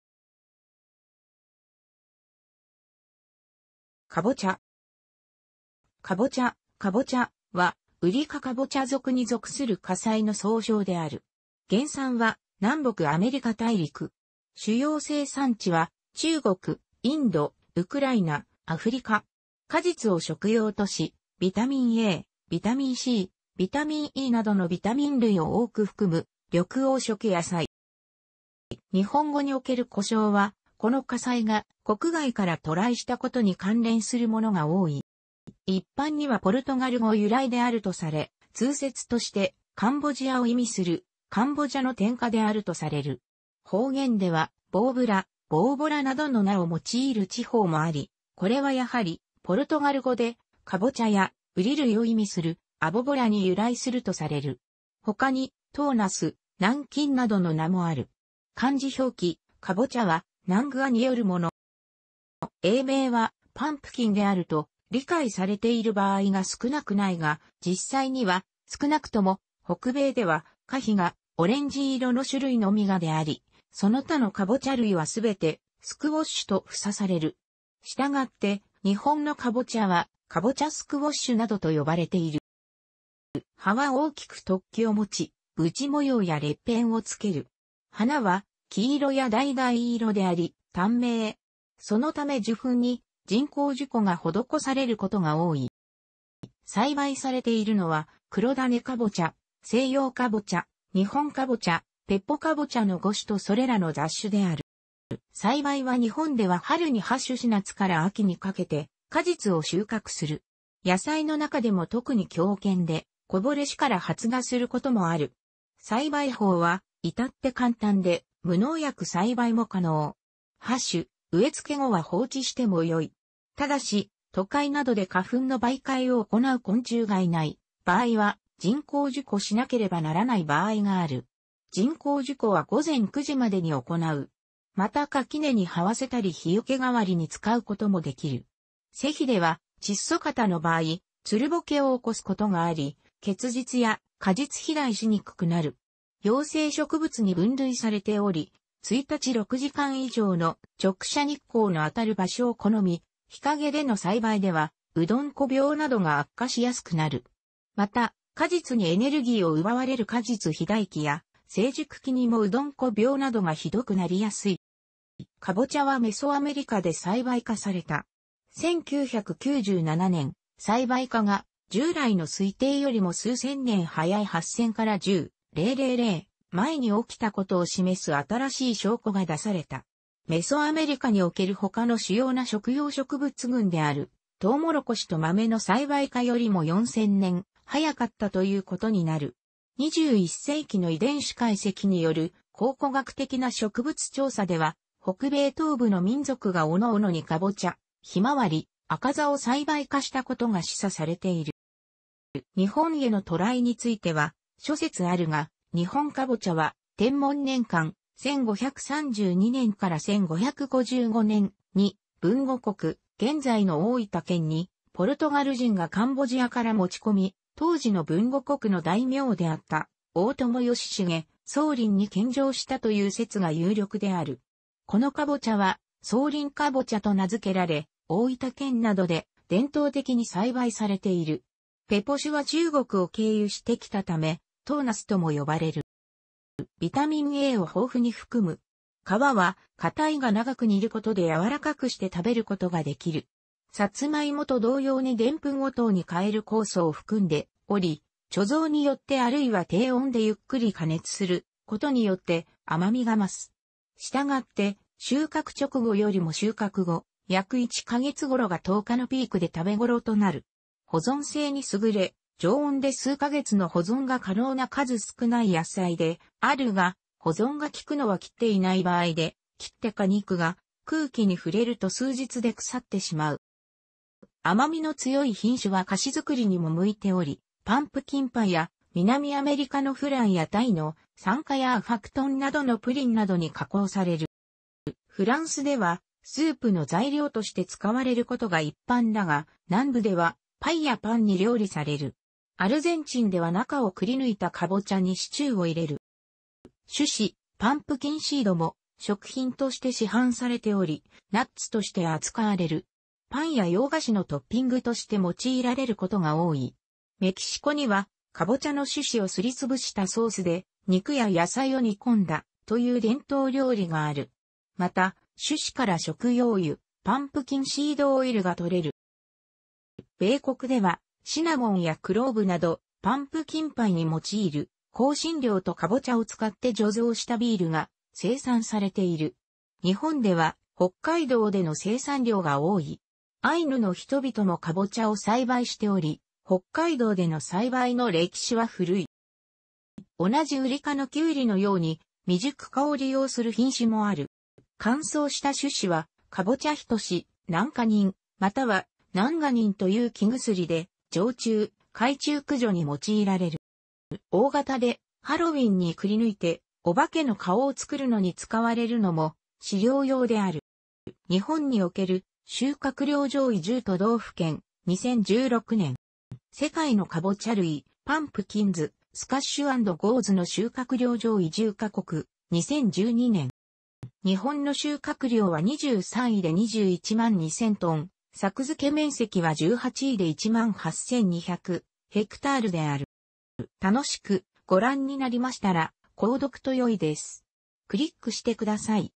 カボチャ
カボチャ（南瓜、、）は、ウリ科カボチャ属に属する果菜の総称である。原産は南北アメリカ大陸。主要生産地は中国、インド、ウクライナ、アフリカ。果実を食用とし、ビタミンA、ビタミンC、ビタミンEなどのビタミン類を多く含む緑黄色野菜。

日本語における呼称は、この果菜が国外から渡来したことに関連するものが多い。

一般にはポルトガル語由来であるとされ、通説として「カンボジア」を意味する （カンボジャ）の転訛であるとされる。方言では「ぼうぶら」「ボーボラ」などの名を用いる地方もあり、これはやはりポルトガル語で、「カボチャ」や「ウリ類」を意味する （アボボラ）に由来するとされる。ほかに「唐茄子（とうなす）」「南京（なんきん）」などの名もある。 漢字表記「南瓜」は （ナングァ; ）によるもの。

英名は （パンプキン）であると理解されている場合が少なくないが、実際には、少なくとも北米では、果皮がオレンジ色の種類のみが であり、その他のカボチャ類は全て （スクウォッシュ）と総称される。したがって日本のカボチャは、 （カボチャ・スクウォッシュ）などと呼ばれている。

葉は大きく突起を持ち、斑模様や裂片をつける。花は黄色や橙色であり、短命。そのため受粉に人工授粉が施されることが多い。

栽培されているのは、、クロダネカボチャ、セイヨウカボチャ、ニホンカボチャ、ペポカボチャの5種とそれらの雑種である。

栽培は日本では春に播種し夏から秋にかけて果実を収穫する。野菜の中でも特に強健で、こぼれ種から発芽することもある。栽培法はいたって簡単で、無農薬栽培も可能。播種・植えつけ後は放置してもよい。ただし、都会などで花粉の媒介を行う昆虫がいない場合は人工授粉しなければならない場合がある。人工授粉は午前9時までに行う。また垣根に這わせたり日よけ代わりに使うこともできる。施肥では窒素過多の場合、つるぼけを起こすことがあり、結実や果実肥大しにくくなる。陽性植物に分類されており、一日6時間以上の直射日光の当たる場所を好み、日陰での栽培ではウドンコ病などが悪化しやすくなる。また、果実にエネルギーを奪われる果実肥大期や成熟期にもウドンコ病などが酷くなりやすい。

カボチャはメソアメリカで栽培化された。1997年、栽培化が従来の推定よりも数千年早い8,000 - 10,000年前に起きたことを示す新しい証拠が出された。メソアメリカにおける他の主要な食用植物群であるトウモロコシと豆の栽培化よりも4,000年早かったということになる。21世紀の遺伝子解析による考古学的な植物調査では、北米東部の民族がおのおのにカボチャ、ヒマワリ、アカザを栽培化したことが示唆されている。

日本への渡来については諸説あるが、ニホンカボチャは天文年間（1532年-1555年）に豊後国（現在の大分県）にポルトガル人がカンボジアから持ち込み、当時の豊後国の大名であった大友義鎮（宗麟）に献上したという説が有力である。このカボチャは「宗麟かぼちゃ」と名づけられ大分県などで伝統的に栽培されている。ペポ種は中国を経由して来たため唐茄子とも呼ばれる。

ビタミンAを豊富に含む。皮は硬いが長く煮ることでやわらかくして食べることができる。サツマイモと同様にデンプンを糖に変える酵素を含んでおり、貯蔵によってあるいは低温でゆっくり加熱することによって甘味が増す。従って、収穫直後よりも収穫後、約1か月頃が糖化のピークで食べ頃となる。保存性に優れ、常温で数か月の保存が可能な数少ない野菜であるが、保存がきくのは切っていない場合で、切って果肉が空気に触れると数日で腐ってしまう。

甘みの強い品種は菓子作りにも向いており、パンプキンパイや、南アメリカのフランやタイの「サンカヤー・ファクトン」などのプリンなどに加工される。

フランスではスープの材料として使われることが一般だが、南部ではパイやパンに料理される。アルゼンチンでは中をくりぬいたカボチャにシチューを入れる。

種子（パンプキンシード）も食品として市販されており、ナッツとして扱われる。パンや洋菓子のトッピングとして用いられることが多い。メキシコにはカボチャの種子をすりつぶしたソースで肉や野菜を煮込んだ、 という伝統料理がある。また、種子から食用油（パンプキンシードオイル）が取れる。

米国ではシナモンやクローブなど、パンプキンパイに用いる香辛料とカボチャを使って醸造したビールが生産されている。日本では北海道での生産量が多い。アイヌの人々もカボチャを栽培しており、北海道での栽培の歴...